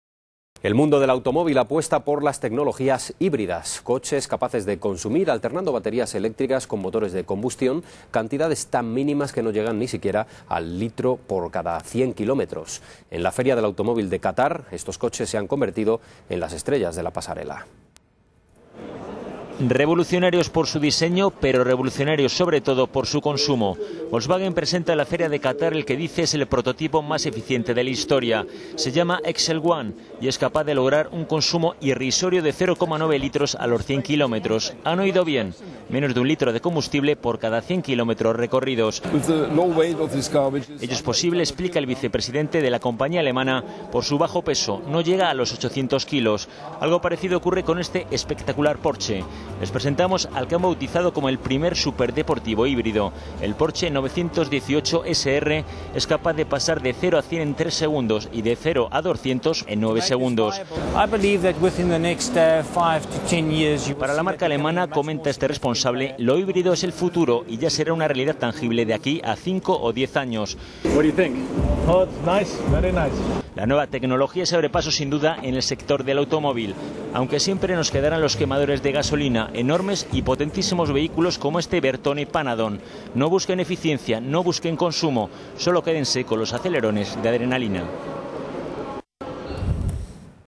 Feria del automóvil de Qatar
Los prototipos más revolucionarios del mercado se muestran en Qatar: la pasarela más exclusiva de automóviles de la temporada de invierno.
Alto rendimiento y ahorro de consumo es ya una realidad...